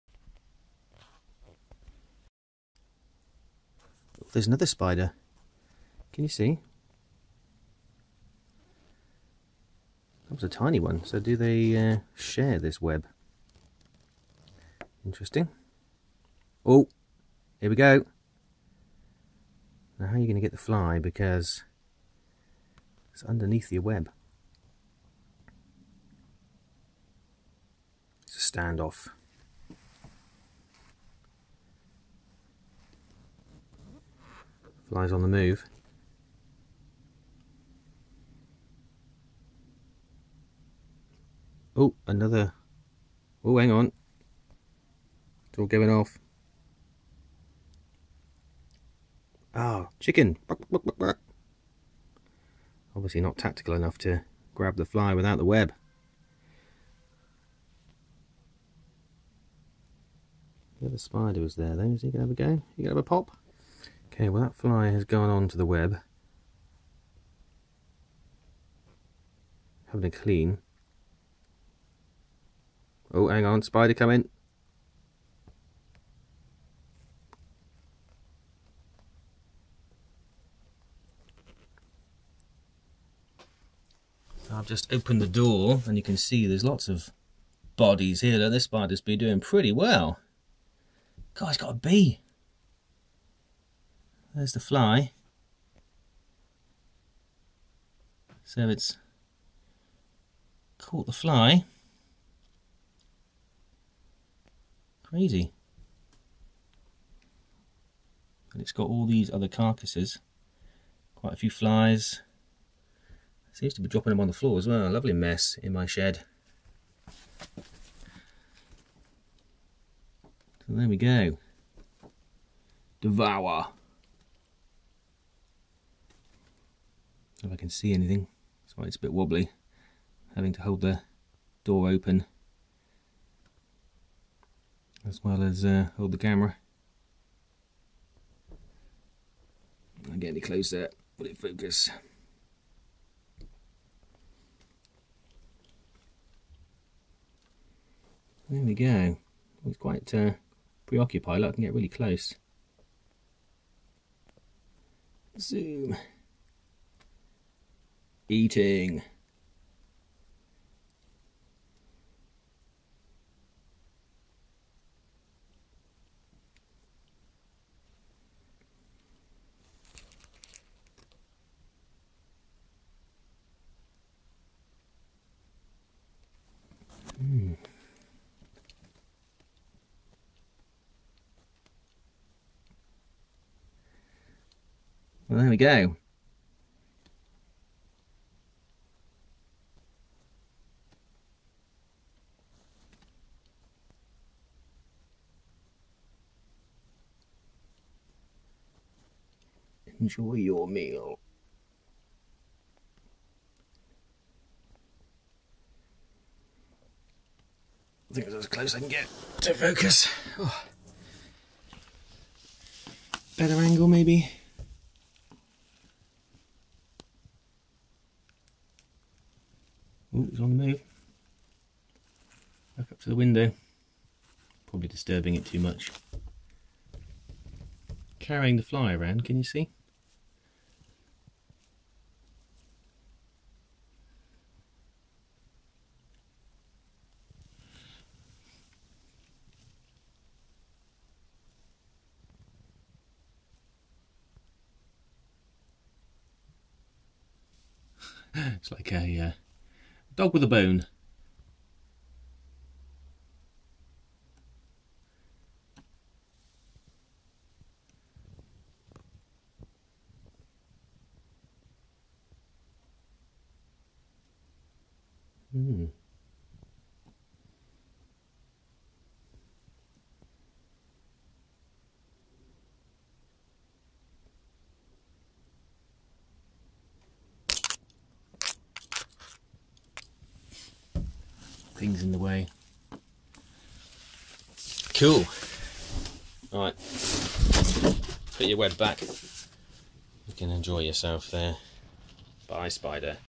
House Spider Catching A Blue Bottle Fly In The Shed
This was a slow but steady capture of the Blue Bottle Fly by the House spider (Tegenaria domestica).
There was also another much smaller spider trying to get in on the action.